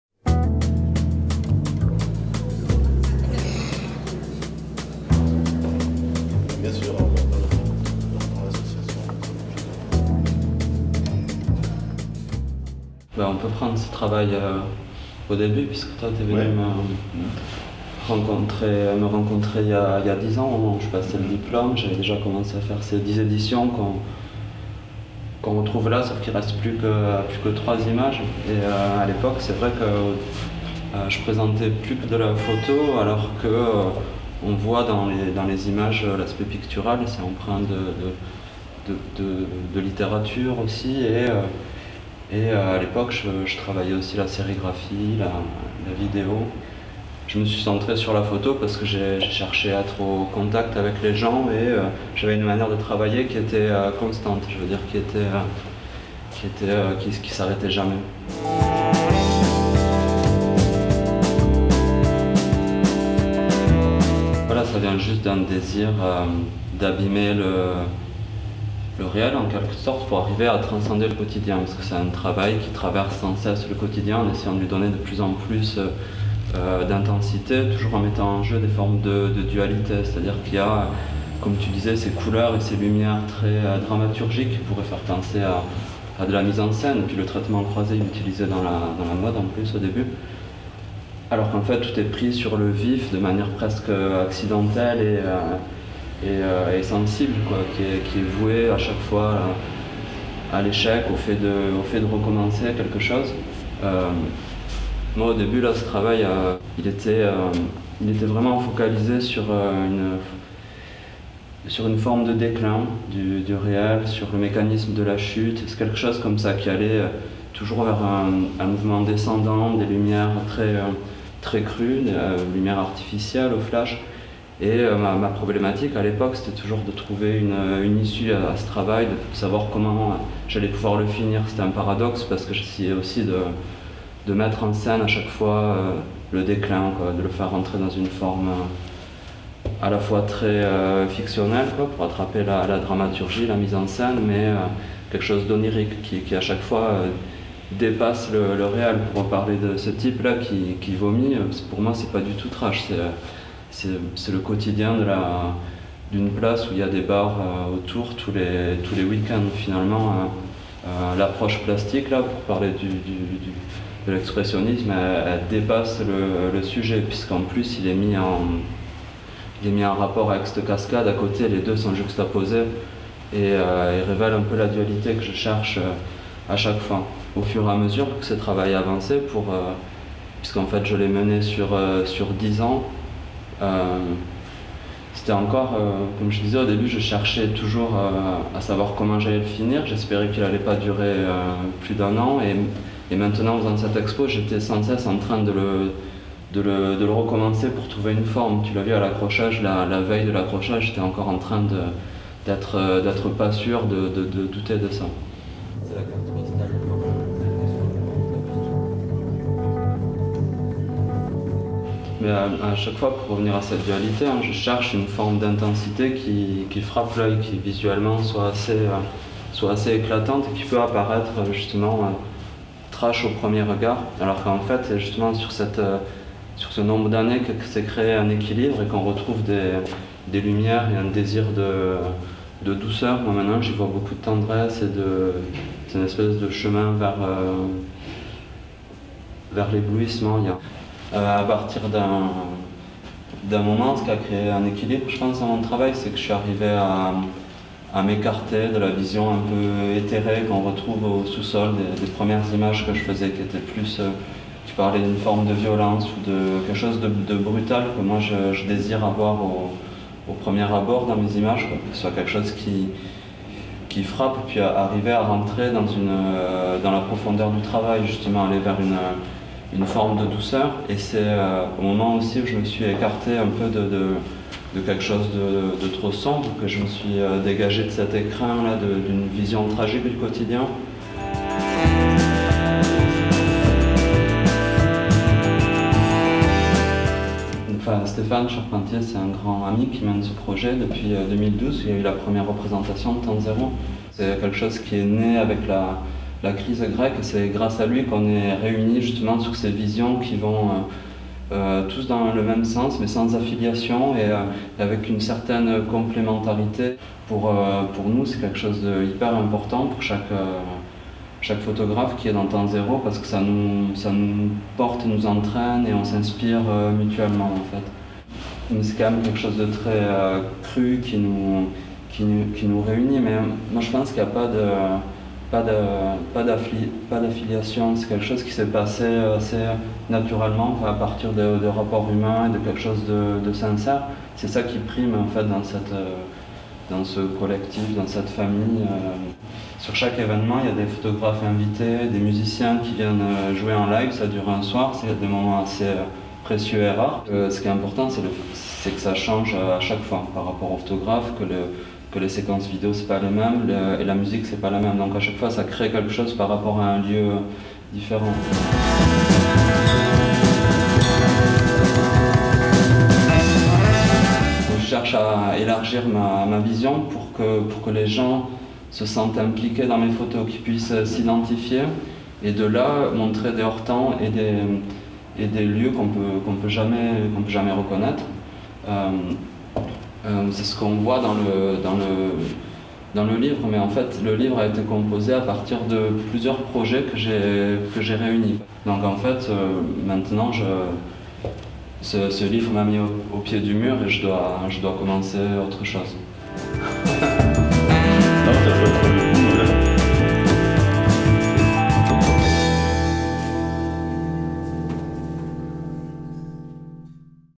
Gaël Bonnefon "Elegy for the Mundane" à la galerie du Château d'eau
Reformulant sans cesse son travail, Gaël Bonnefon puise dans son corpus quotidiennement récolté pour nous proposer cette exposition.